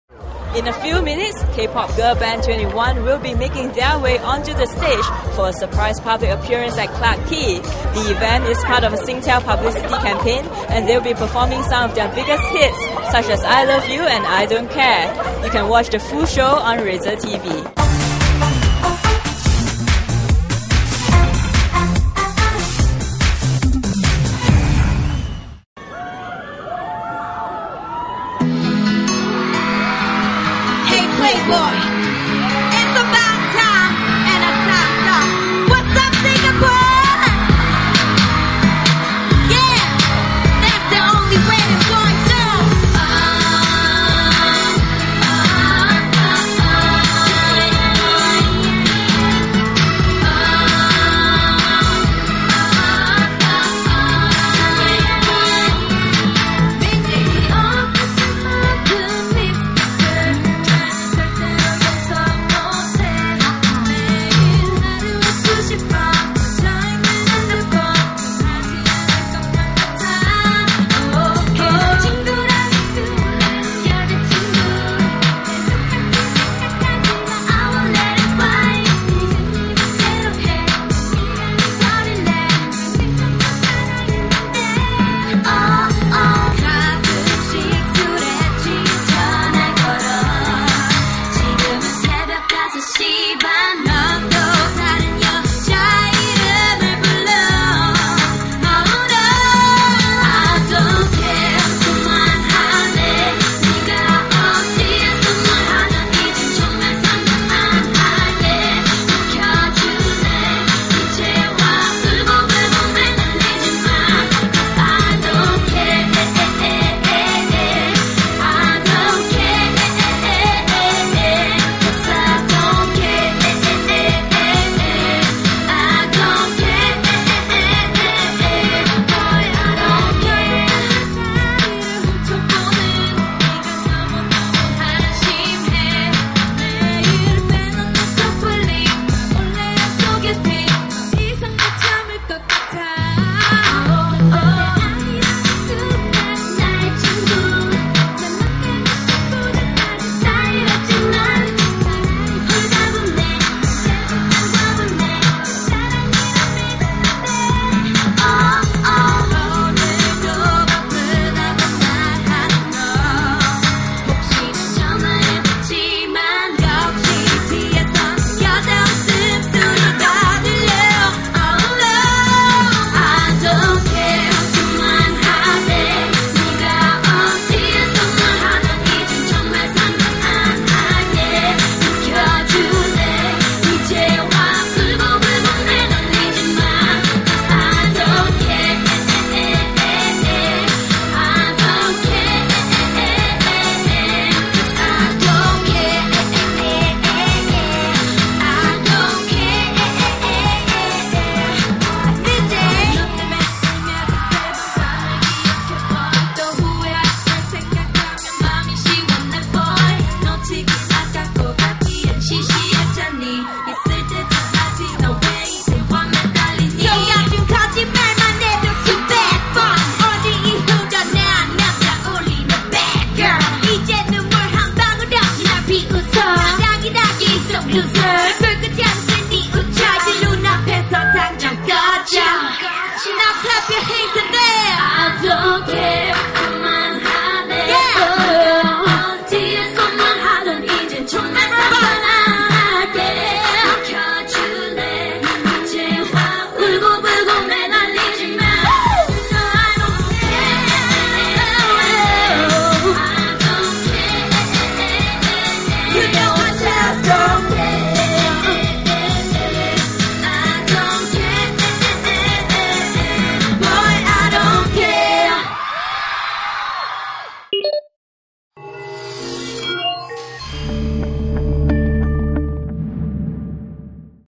2NE1 springs surprise performance at Clarke Quay
(2NE1 showcase Pt 1)K-pop girl group 2NE1 staged a surprise public performance at Clarke Quay on Friday evening, one month ahead of their first ever concert here in Singapore, much to the delight of hundreds of fans.